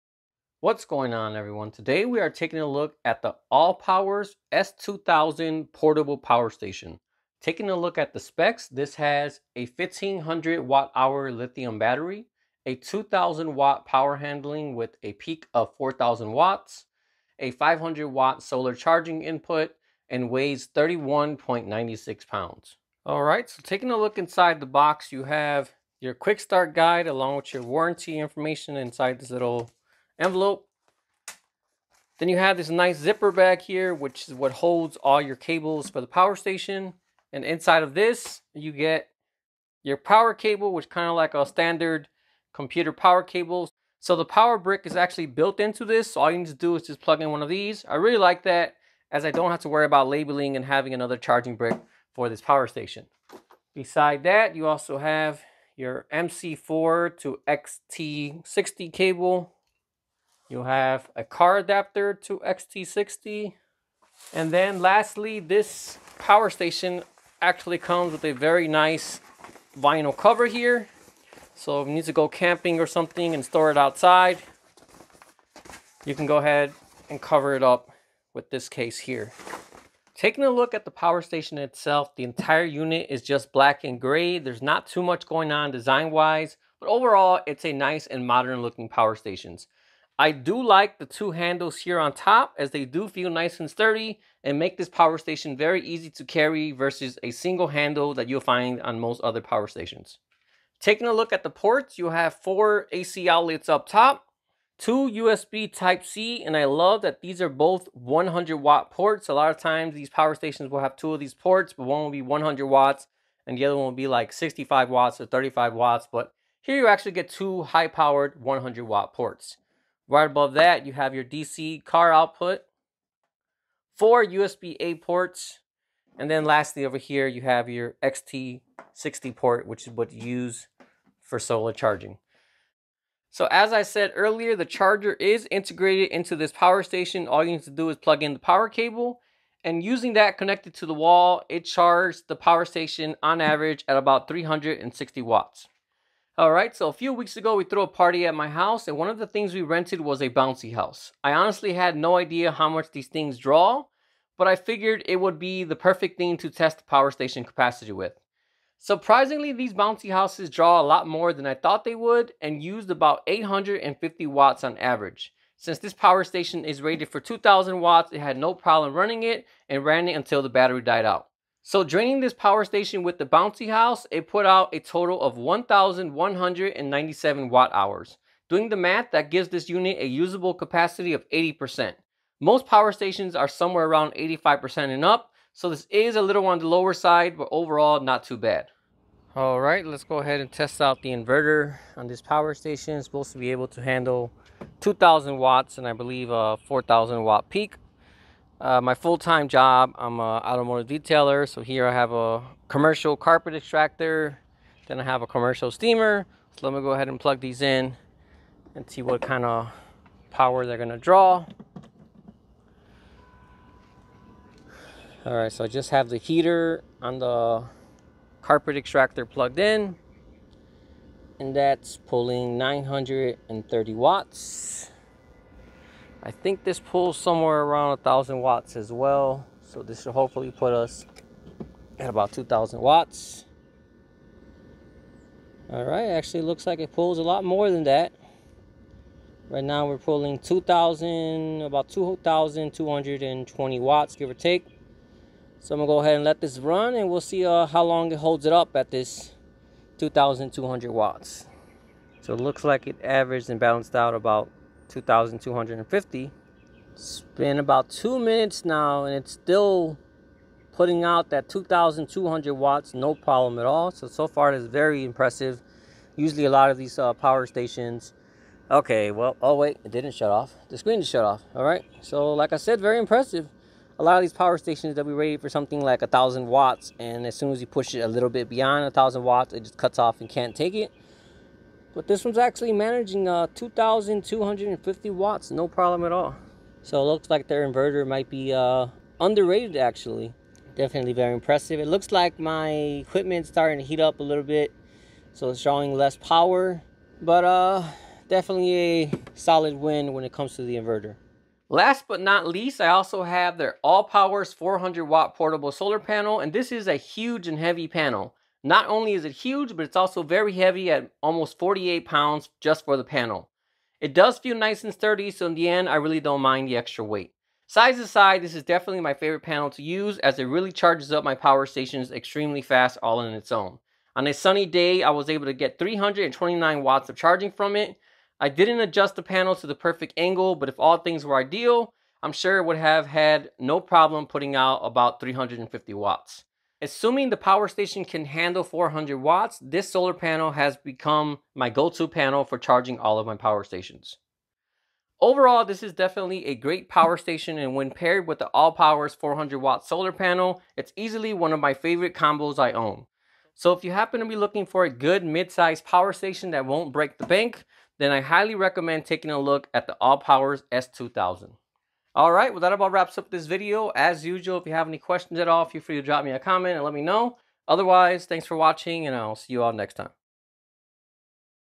AllPowers S2000 Portable Power Station Review
Today we are taking a look at the “AllPowers S2000” Portable Power Station. This power station has a 1500Wh capacity, a 2000 watt power handling, and a 500w solar charging input. Watch to find out more details and see how it performed!

400 Watt Solar Panel